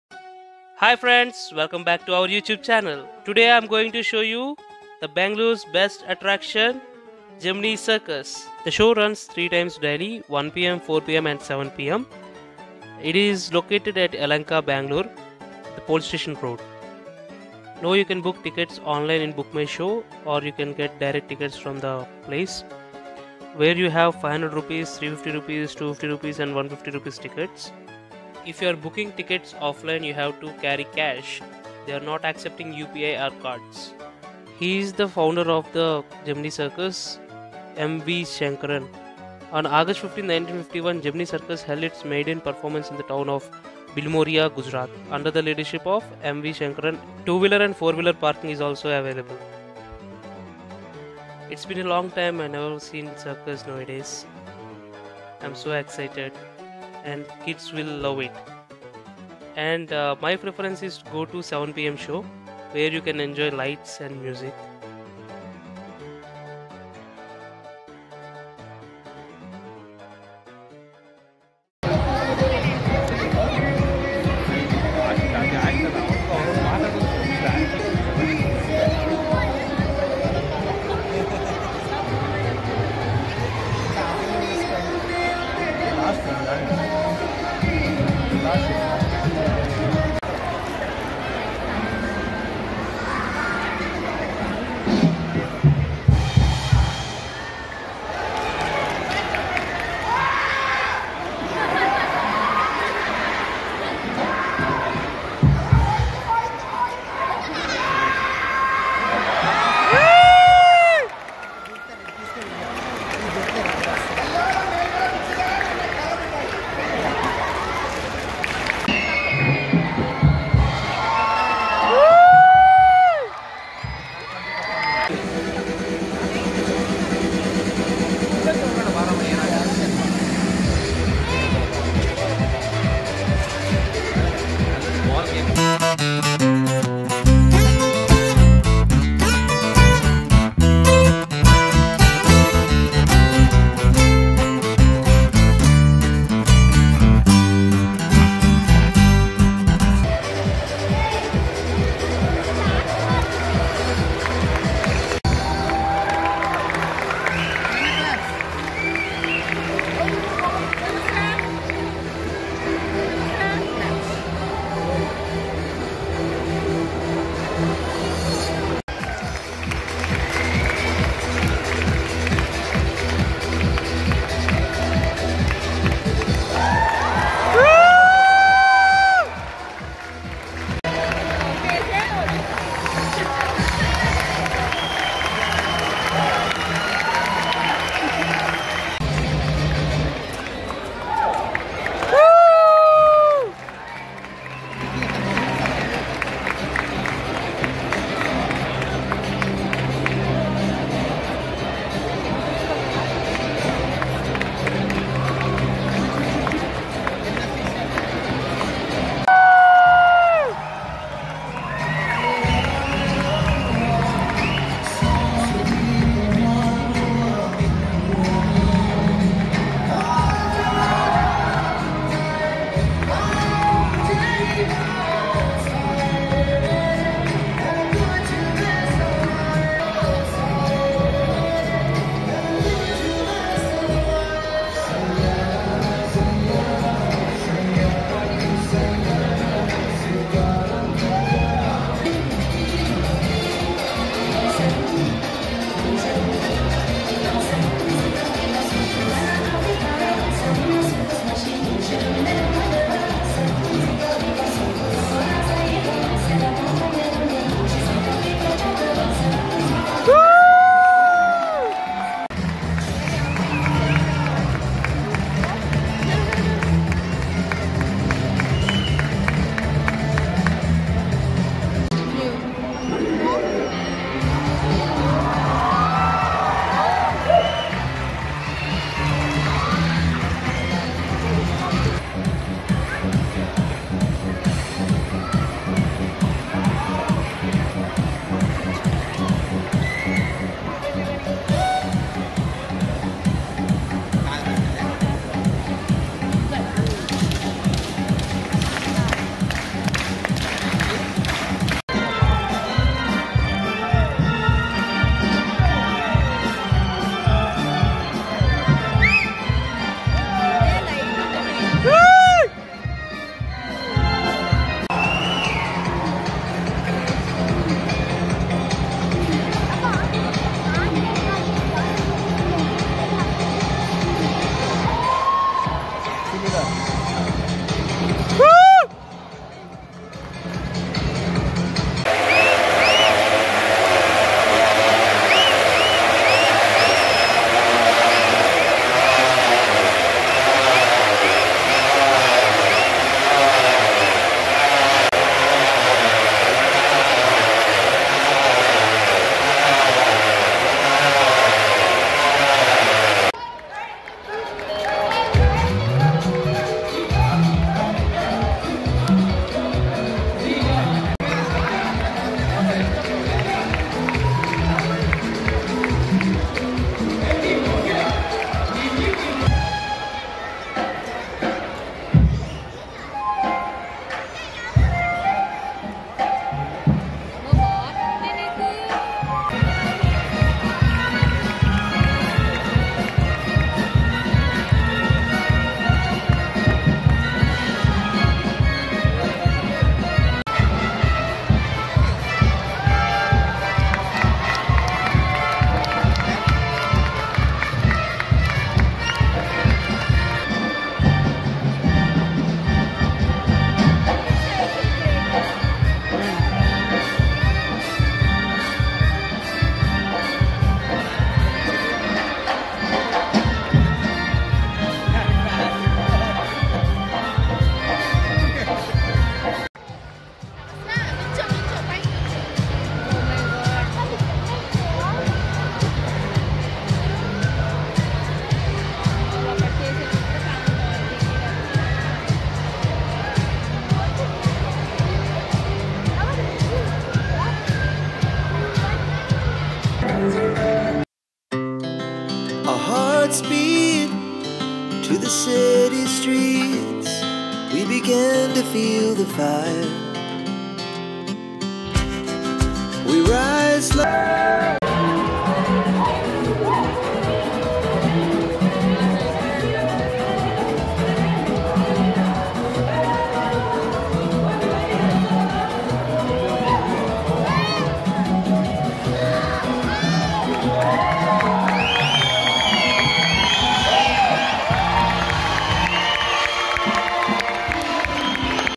Gemini Circus in Yelahanka Bangalore 2024 info
Performancers from around the world

Gemini Circle Rs 150 , 250, 350, 500 (Front row 500) | Daily 3 Shows- 1pm 4pm & 7pm.
Note:-Children over three years of age must have a full ticket.

Food stall, 2  and 4 weeler parking availble.
They prefer only cash over UPI or cards.

Note: No animials involved.

Address:
Allalasandra Main Rd, Ambedkar Colony, Yelahanka New Town, Bengaluru, Karnataka 560065